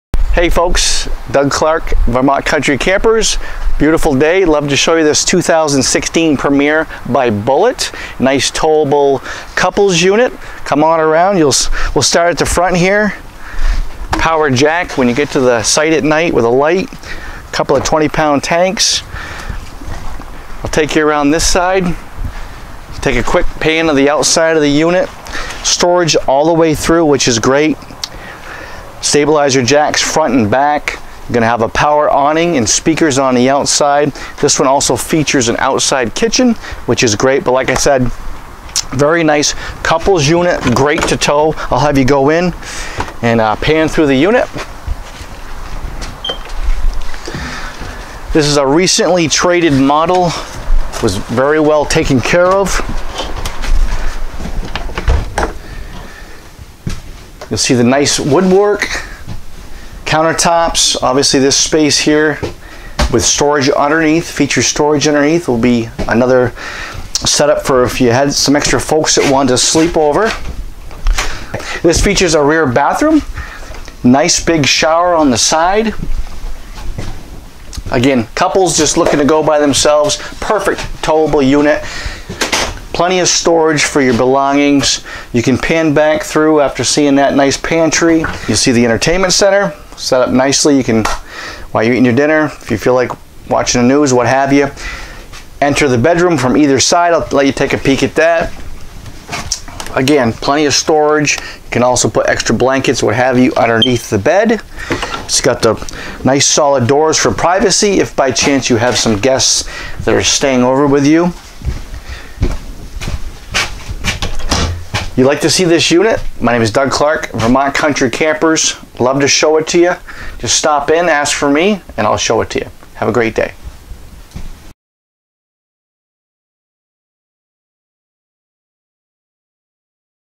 Used 2016 Keystone RV Premier Ultra Lite 22RBPR with Doug Clark
Enjoy camping in this Keystone Premier Ultra Lite travel trailer 22RBPR. This unit features a single slide, rear bath, and outdoor kitchen.

Step inside near the front bedroom with queen size bed. There are wardrobes on both sides of the bed, an overhead cabinet, and a convenient laundry chute.

The living room entertainment center is just off the foot of the queen bed and there are pocket doors that can be closed on either side of the entertainment cabinet for more privacy in the bedroom at night.

There is a u-shaped dinette slide across from the Premier kitchen which features a large sink, three burner range, and refrigerator. There is also a pantry just off to the side of the dinette for more storage.

The rear bath provides a 30" x 36" shower with glass door, vanity with sink and toilet. There is also a linen cabinet next to the toilet for storage.

Outside enjoy the pass through storage compartment up front for all of your exterior camping gear and more. There is a rear outdoor kitchen that features and refrigerator, sink, and swing out cook center with two burner stove, and so much more!